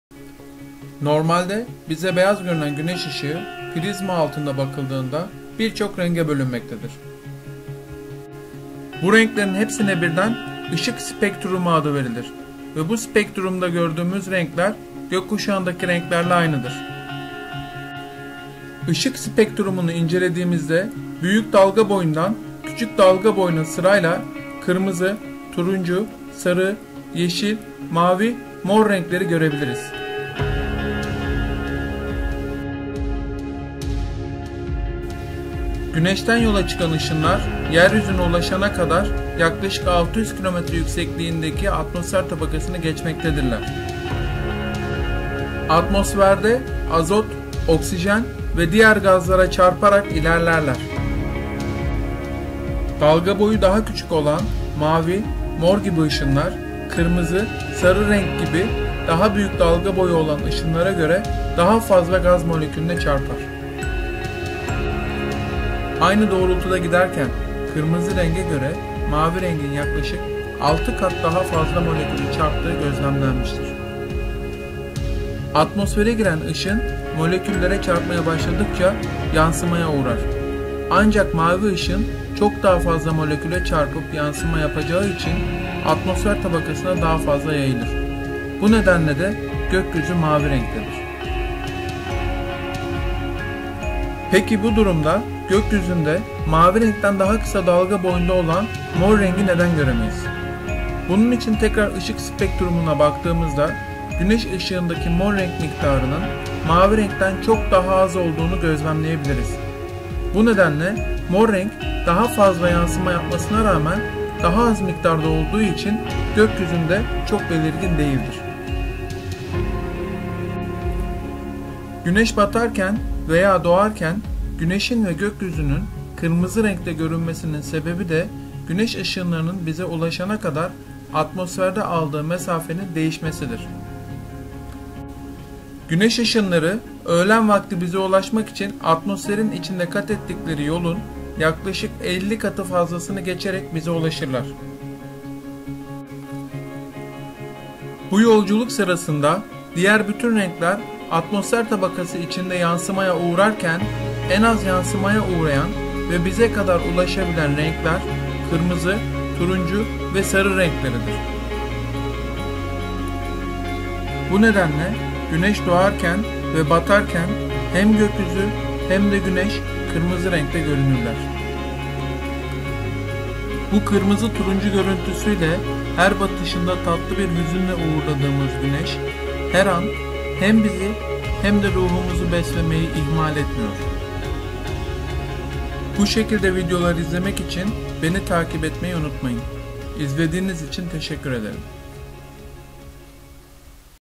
Gokyuzu neden mavidir? Gunes neden kızarır?
Gunesin dogarken ve batarken neden kırmızlastıgı ve gokyuzunun renginin neden mavi oldugunu acıklayan calısmamdır. İyi seyirler